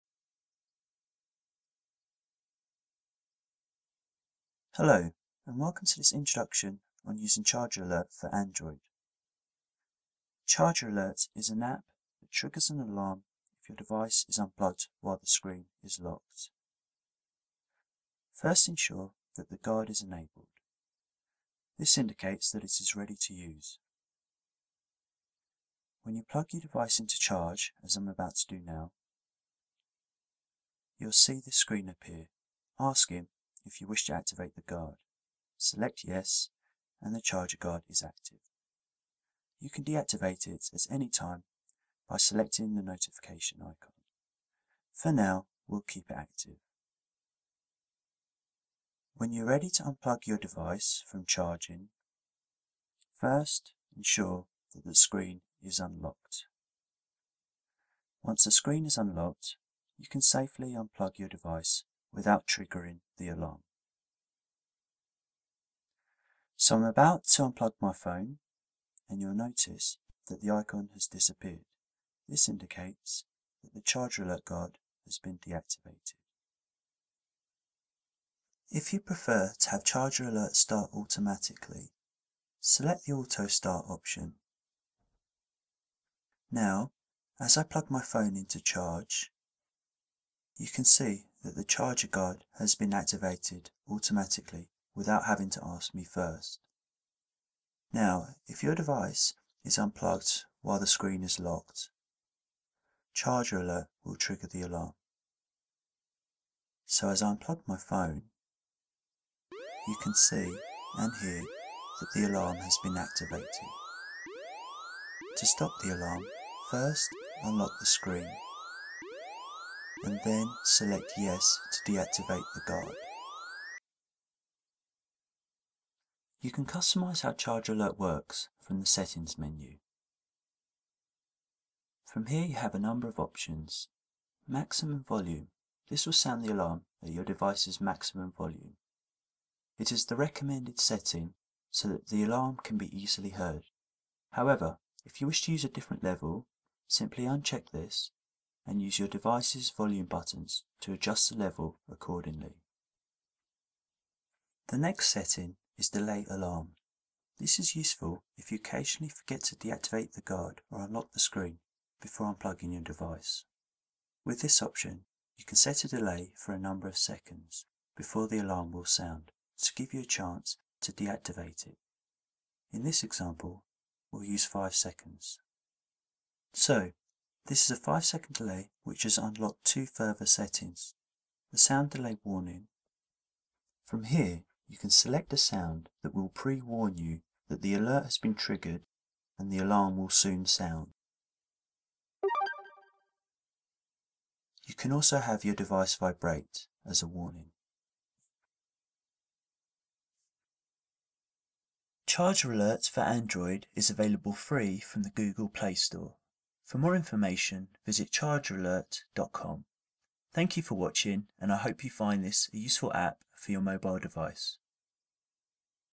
Charger Alert app for Android
An introduction to using Charger Alert v1.0 for Android. Charger Alert is an app that warns you with an alarm, if somebody unplugs your device while it is charging.